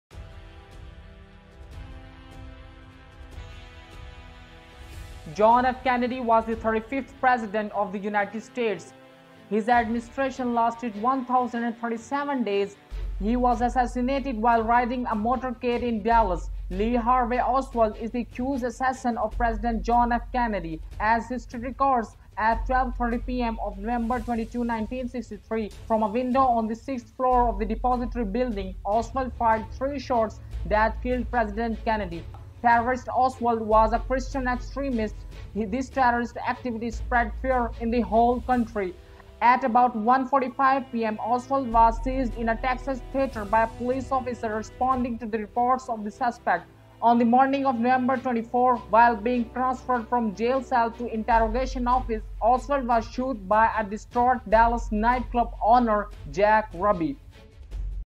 John F Kennedy assassination's | Lee Harvey Oswald | Complete Assassinations story
This video will give you the information of John F  Kennedy assassination's by  Lee Harvey Oswald. This is a Complete Assassinations story..